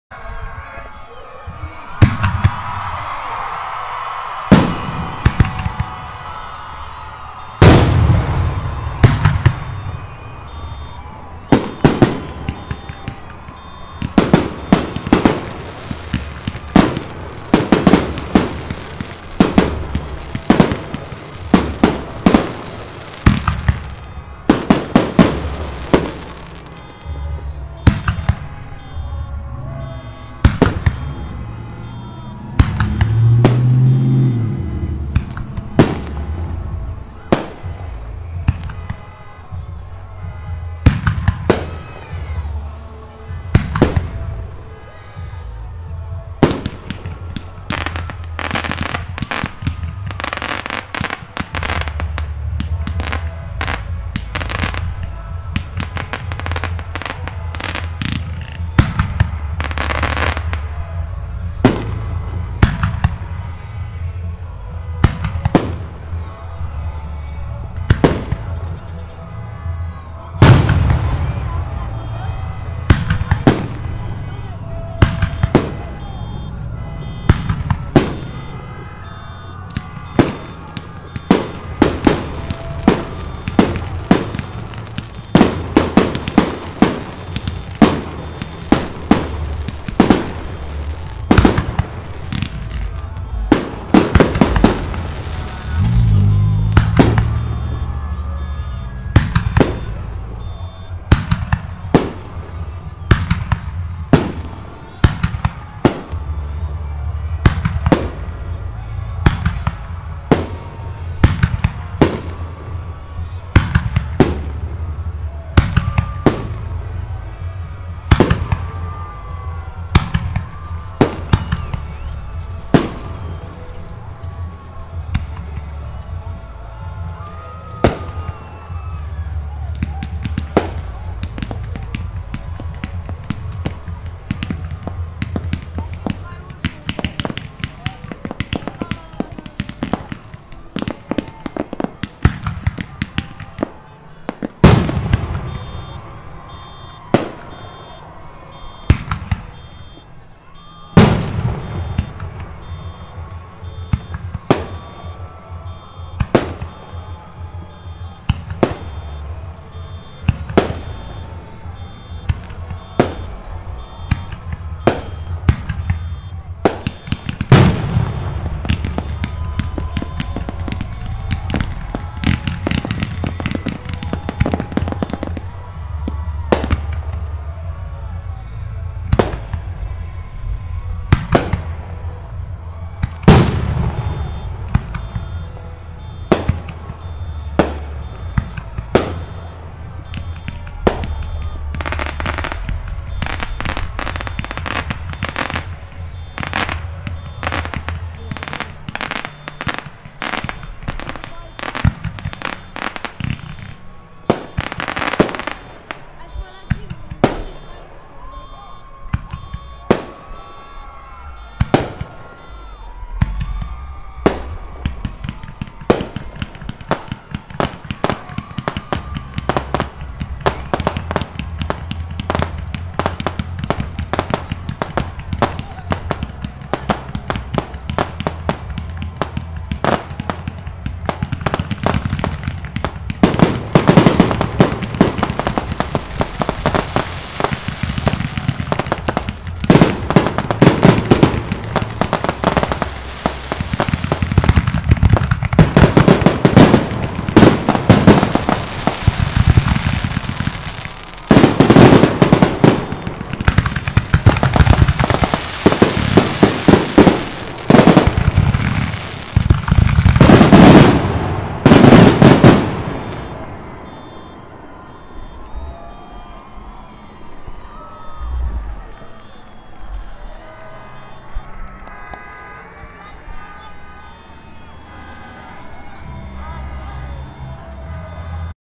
Dinuba High School Graduation Fireworks 2014
Fireworks show at the end of the graduation ceremony at Dinuba High School, May 29,2014. Audio must be played at maximum volume through a good set of speakers with deep bass response.